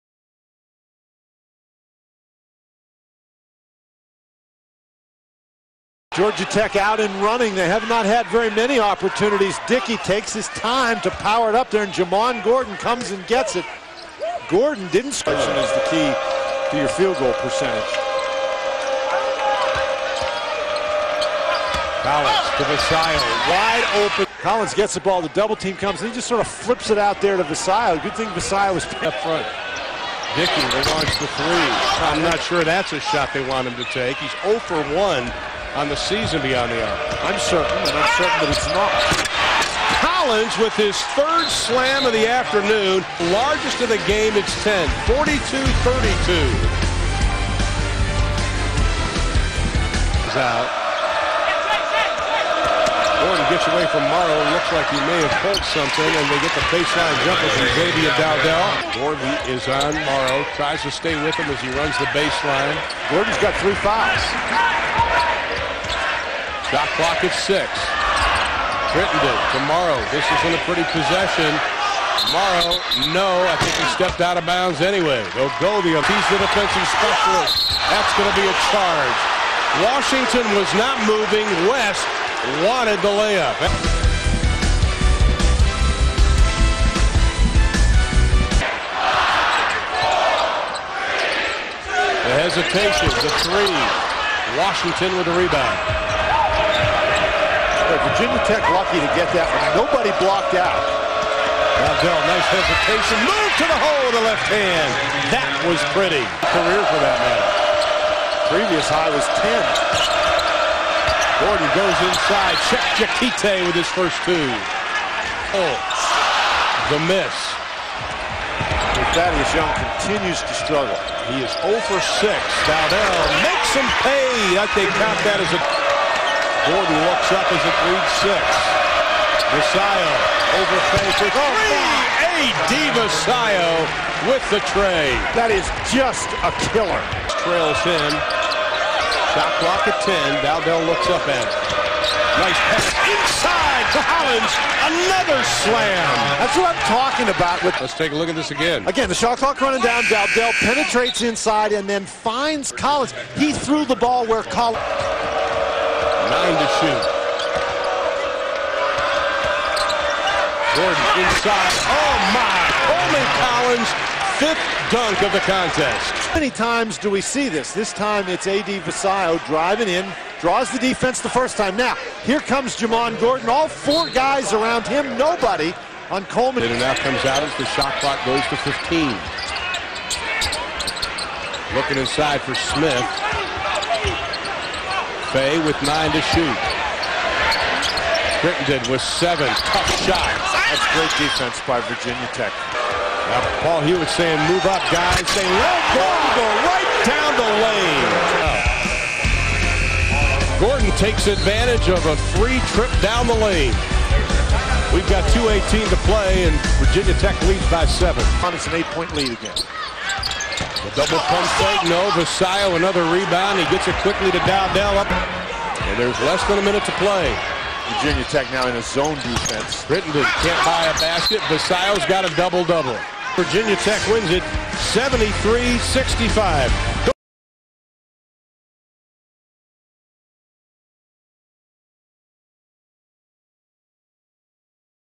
Virginia Tech-Georgia Tech Basketball 1.28.07 | 2nd Half
Hokies wallop jackets in hotlanta